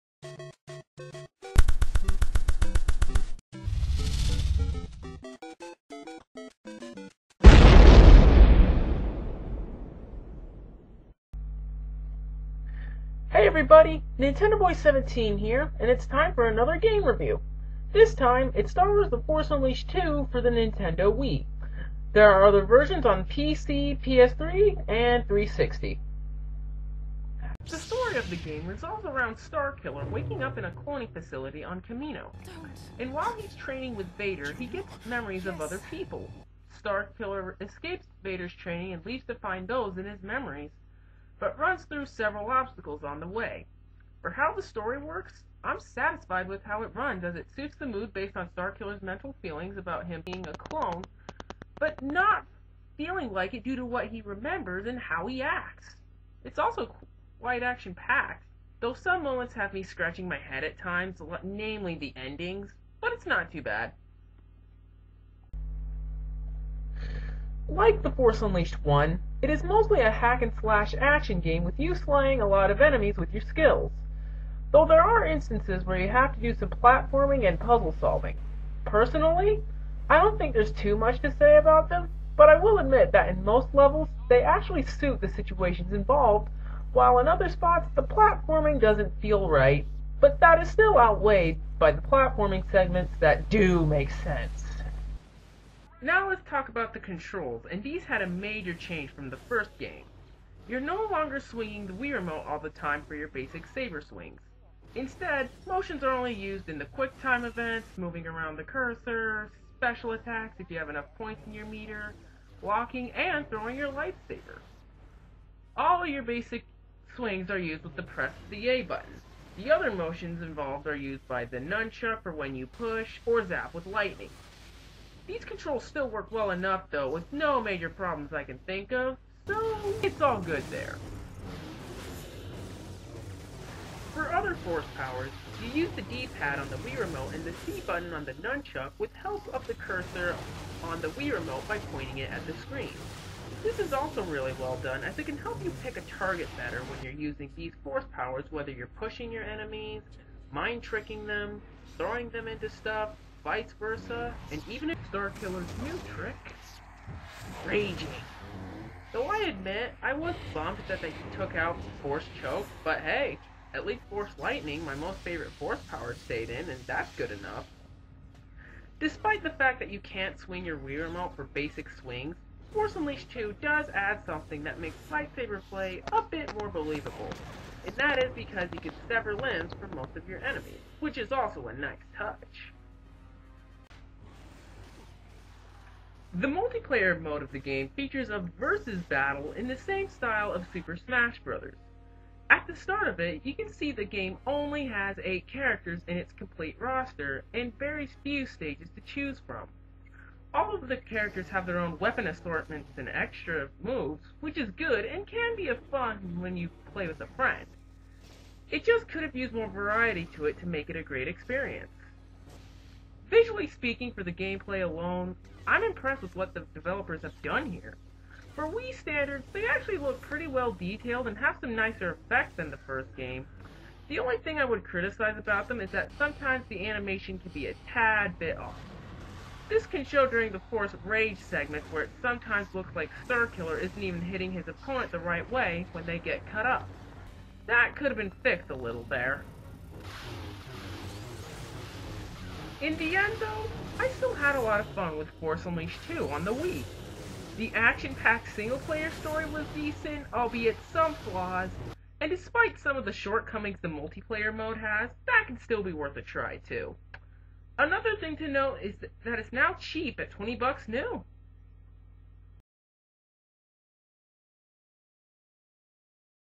nintendoboy17 reviews Star Wars: The Force Unleashed II (Nintendo Wii version)
Oh MAN, it's been a while since I made one of these. This was going to come out sometime near the end of March or beginning of April, but something got in the way.

Anyway, hope you enjoy the review.

Star Wars: The Force Unleashed II

Publisher: Lucasarts
Developer: Red Fly Studios (Wii version)
Version reviewed: Nintendo Wii
Also on: PS3, XBOX 360, PC